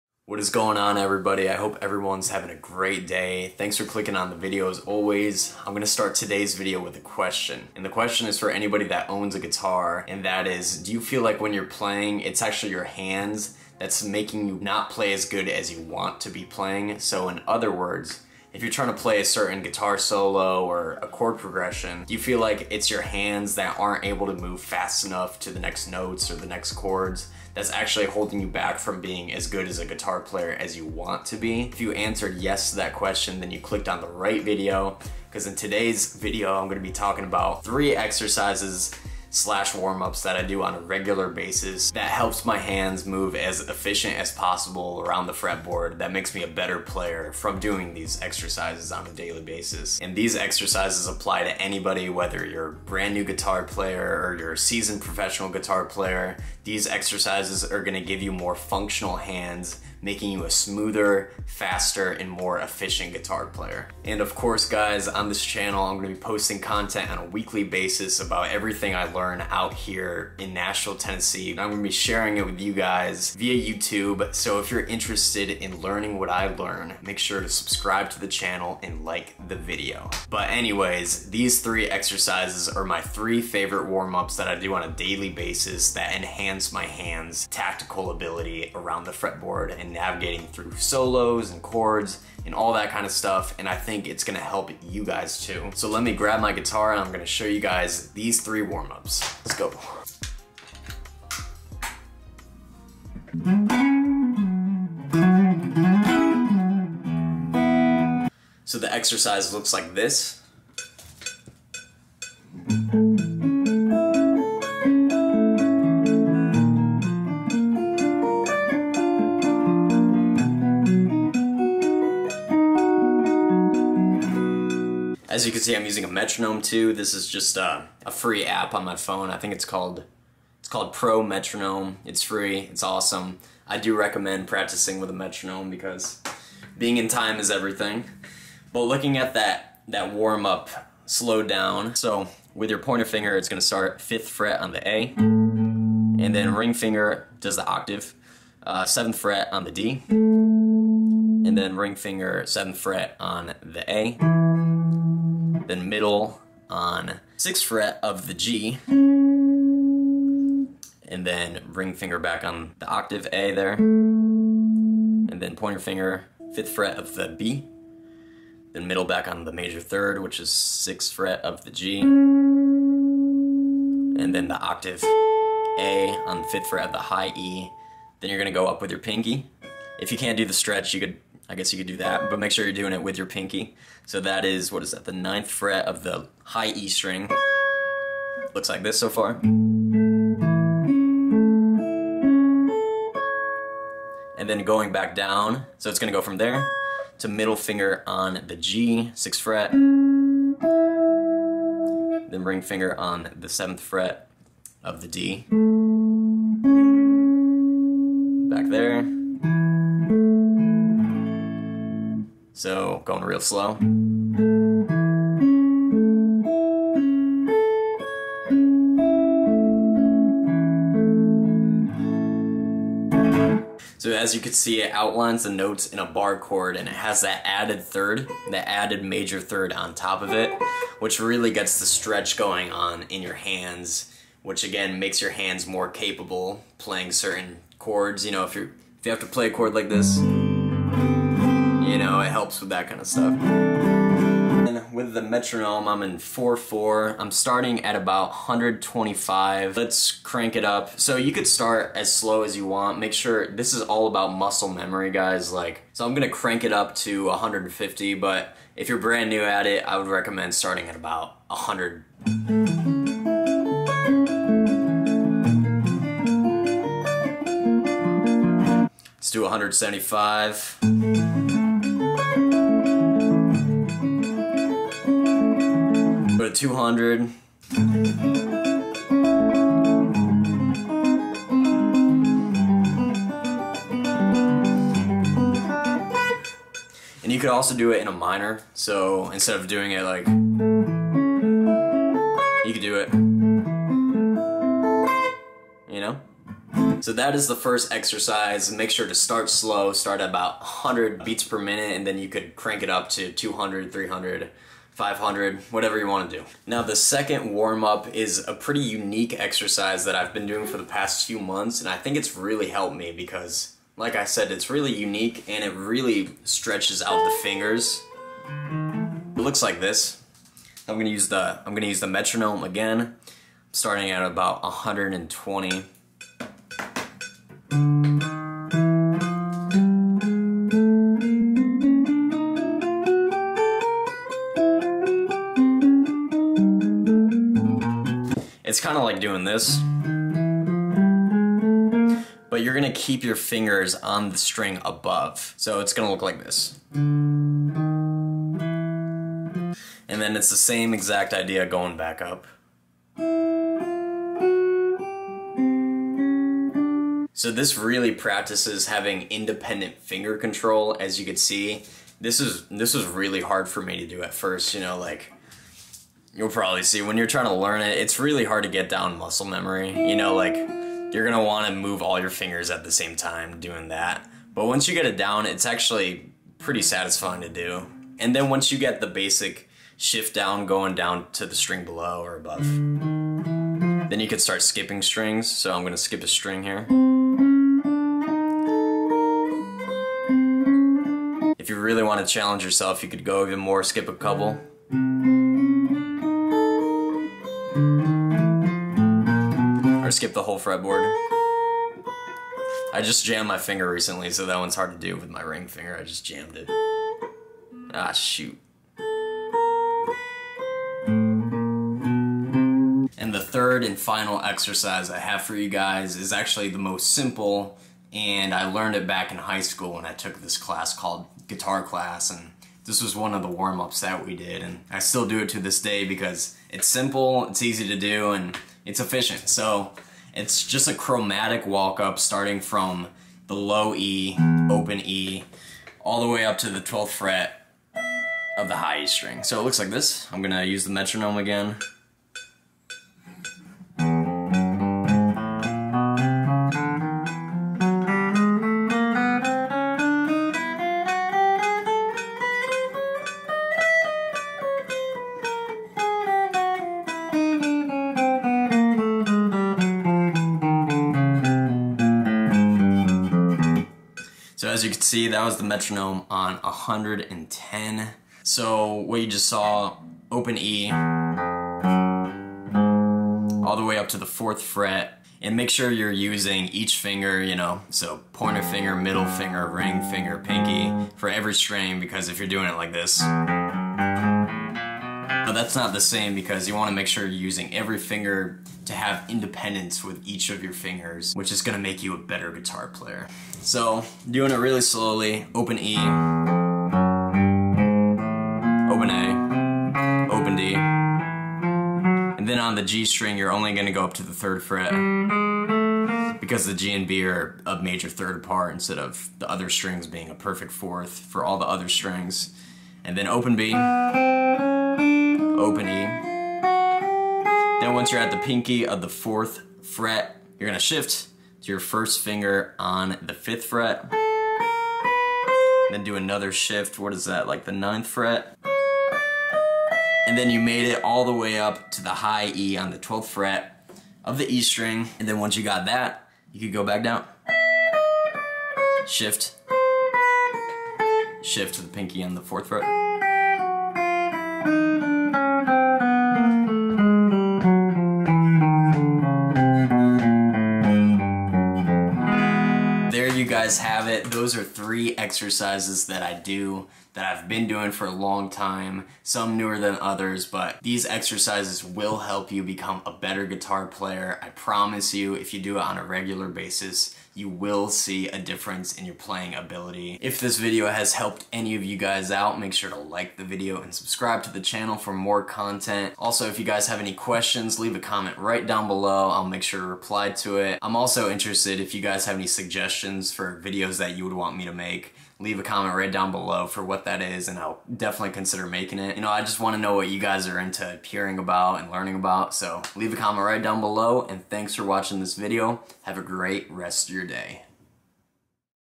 Three Guitar Warmups that will Exponentially Increase Playing Capability (With Consistency)
Whats up guys! In todays video, I am showing you three of my favorite warmups that I do that keep my hands at peak performance for playing shows, recording guitar, or just practicing at home.  These exercises have also made me a better guitar play by challenging the flexibility of my fingers while keeping time doing these warmups. So what you could expect from doing these practices on a regular basis would be:  More capable hands for smoother chord transitions and smoother player in general, increased sense of time while playing, more flexible hands, and fingers that are capable of working independently.  I would also include doing forearm/hand stretching to get the most out of these practices.  Hope this video helps!